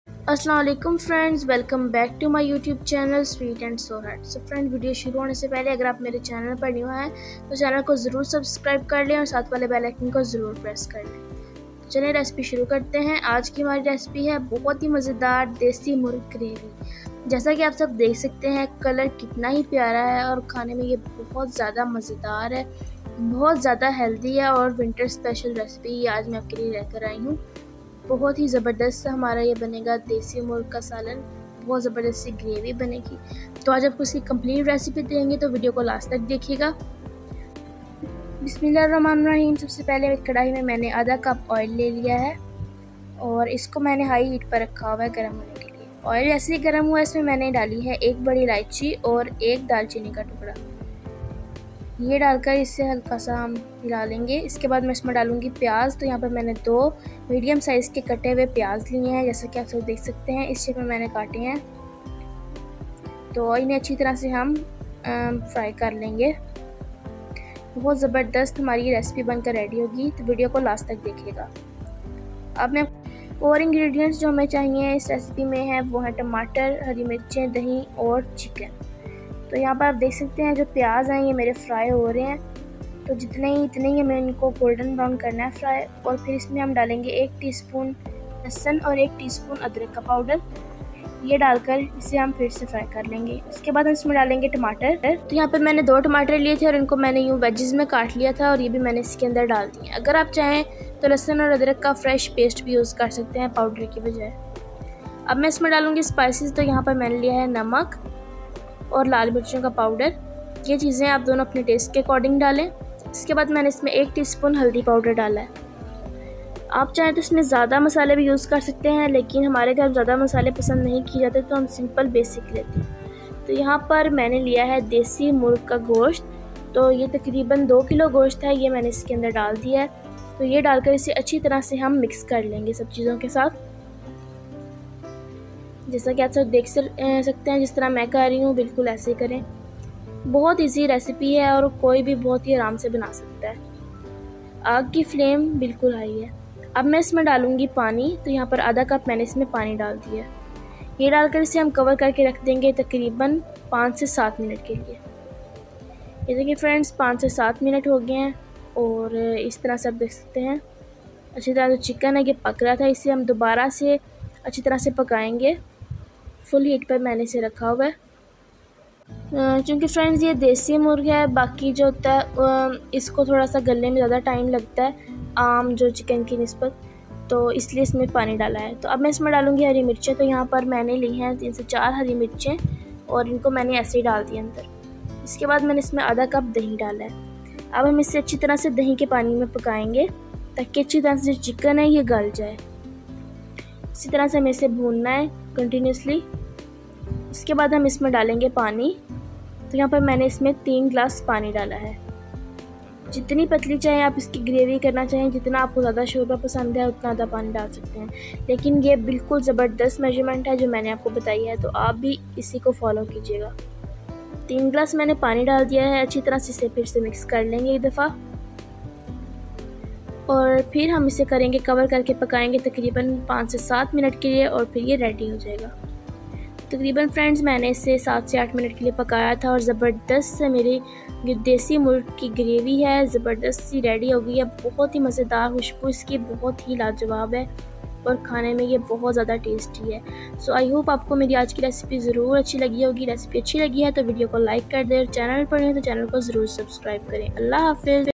Dase Murgh Gravy Recipe in Urdu | Best recipe for making dase murgh shorba.
Today's recipe is so simple and easy.
It's specially a biggeners recipe.